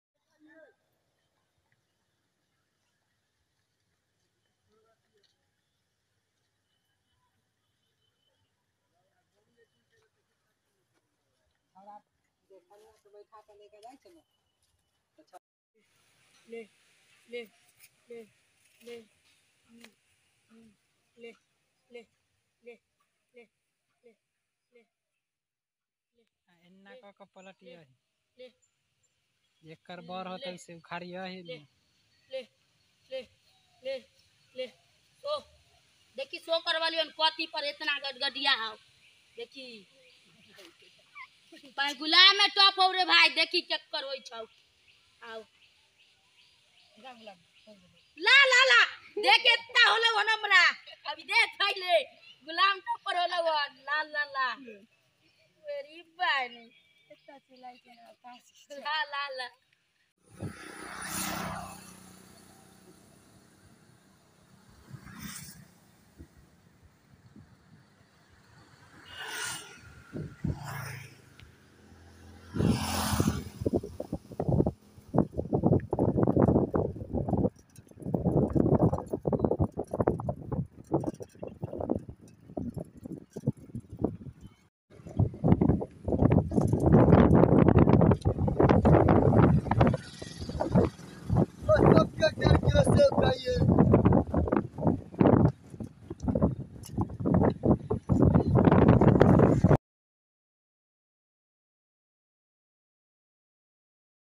Some moment of village. Village life scene.
Is video me aap village life ka scene dekhenge.
गांव का दृश्य।